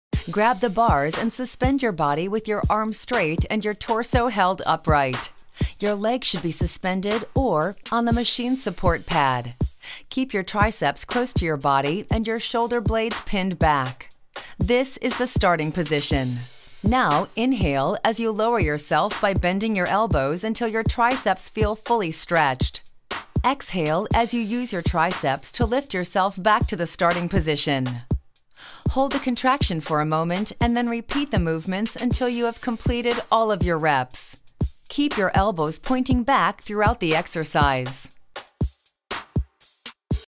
Parallel Bars Dips - Triceps Exercise
(Starting Position)

- Grab the bars and suspend your body with your arms straight and your torso held upright.

- Your legs should be suspended or on the machine’s support pad.

- Keep your triceps close to your body and your shoulder blades pinned back. This is the starting position.

(First Movement)

- Now, inhale as you lower yourself by bending your elbows until your triceps feel fully stretched.

(Second Movement)

- Exhale as you use your triceps to lift yourself back to the starting position.

- Hold the contraction for a moment and then repeat the movements until you have completed all of your reps.

(General Tips)

- Keep your elbows pointing back throughout the exercise.